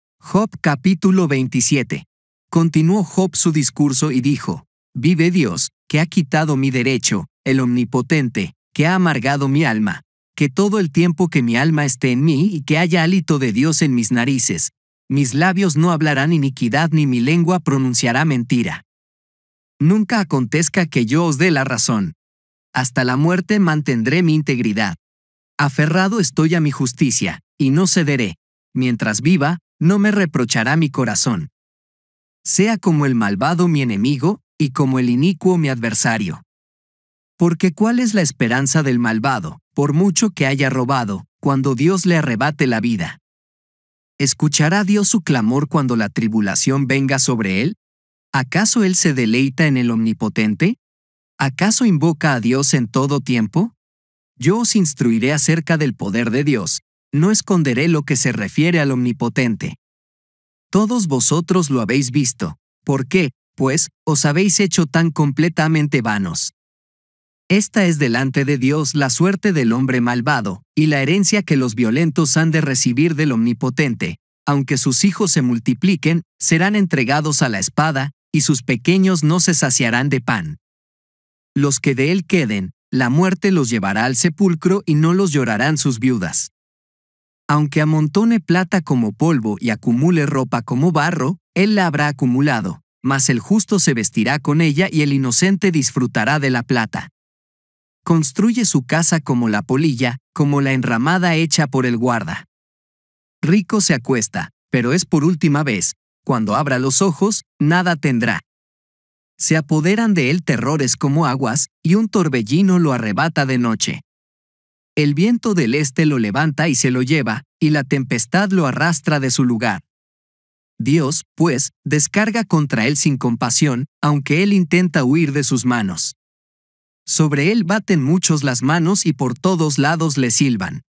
JOB Capitulo 27, una historia de fe, justicia y redención.
La historia de Job es un relato antiguo que ha inspirado a muchas personas a través de los siglos. Job era un hombre justo y piadoso que sufrió grandes pérdidas y enfermedades, pero nunca perdió su fe en Dios. En este video, exploramos la historia de Job y sus lecciones sobre paciencia, sufrimiento, fe y confianza en Dios. Descubre cómo la bondad, la misericordia y la omnipotencia de Dios se manifiestan en medio del sufrimiento y cómo Job encontró restauración y bendición después de sus pruebas.

Únete a nosotros mientras exploramos las enseñanzas de la historia de Job y cómo podemos aplicarlas a nuestras vidas. En este video, descubrirás la sabiduría y el propósito detrás del sufrimiento, la importancia de la humildad y la piedad en la vida cristiana, y cómo podemos encontrar esperanza y restauración en medio de las pruebas de la vida.

¡Gracias por unirte a nosotros en este viaje de fe y aprendizaje!